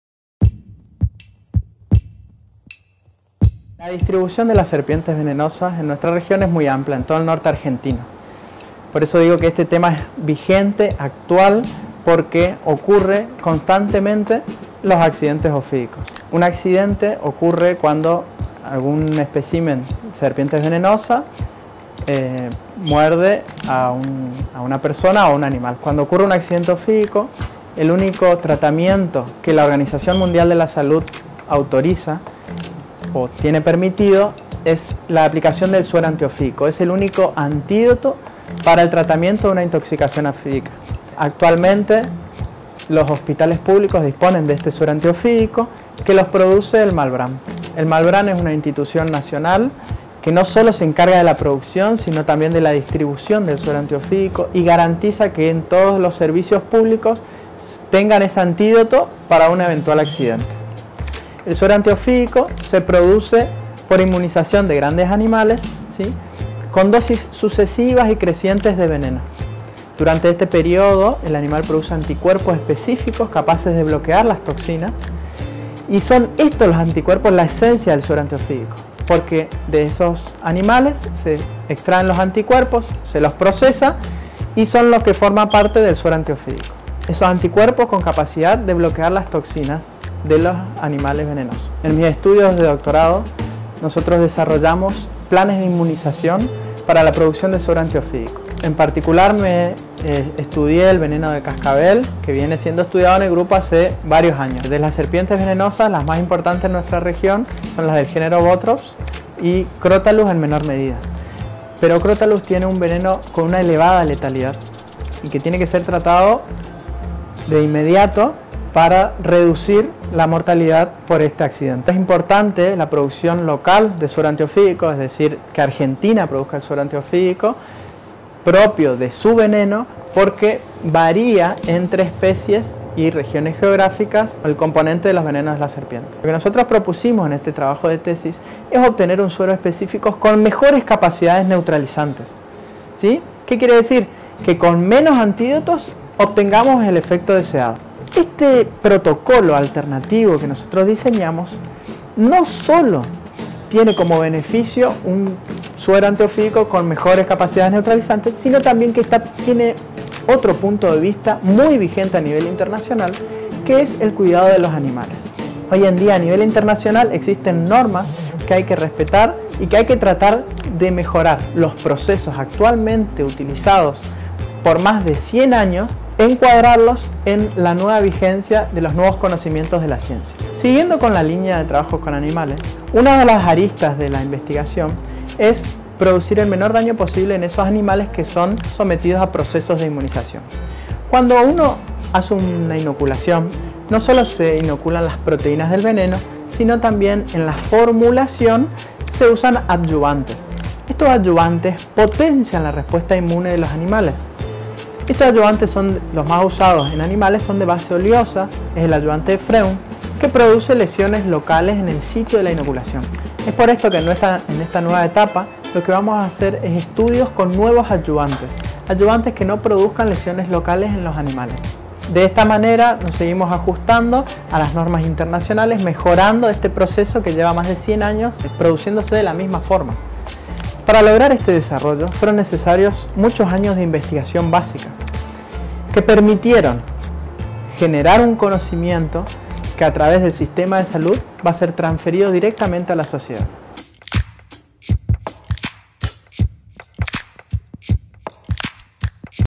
Desarrollan antídotos más efectivos para los envenenamientos por serpientes.
Para la Organización Mundial de la Salud (OMS), una de las enfermedades tropicales desatendidas más prioritarias es el envenenamiento provocado por las mordeduras de serpientes. Los datos epidemiológicos indican que en Argentina se registran unos 400 accidentes ofídicos por año, que pueden causar distintos tipos de parálisis, amputaciones y hasta la muerte.

La administración de suero antiofídico, un material que se obtiene a partir de proteínas específicas presentes en los venenos de cada especie, es el único tratamiento recomendado para neutralizar la acción letal de estos reptiles. En Corrientes, un investigador del CONICET en el Instituto de Química Básica y Aplicada del Nordeste Argentino (IQUIBA - NEA, CONICET - UNNE), está desarrollando técnicas para mejorar la efectividad de estos productos, a través de procedimientos más rápidos, económicos y seguros para los animales productores.

Video: Silvana Siviero (CONICET Nordeste)
Nota: Cecilia Fernandez Castañón. (CONICET Nordeste)